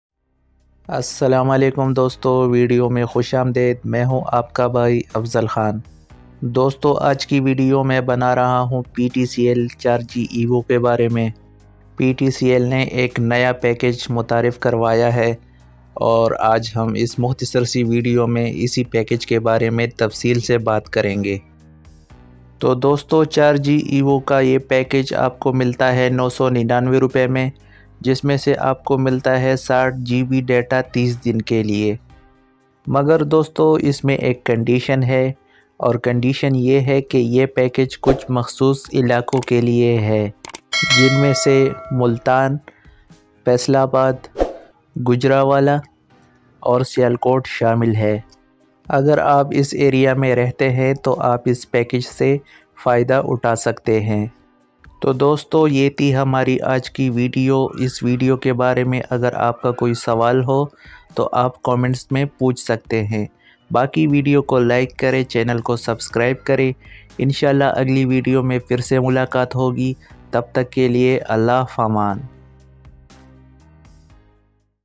Ptcl Charji Evo New Package Rs 999 | Internet Device | Afzal Khan 360
CharJi 999 Offer is valid for Multan, Gujranwala, Faisalabad ; Sialkot regions.

Get your;CharJi;Package*;now for;Rs. 999/-;per month with;60GB;data volume.;Monthly line-rent is;inclusive of all taxes;with no hidden charges.;
Enjoy an amazing Internet experience that allows;endless downloading, streaming, socializing ; gaming.;

PACKAGE DETAILS;

CharJi Device Price:;Standard (CharJi Cloud Rs. 2,500 / CharJi Wingle Rs. 1,500)

Package Volume:;(60 GB)

Package Charges:;Rs. 999 (inclusive of all taxes)

Package Validity:;30 Days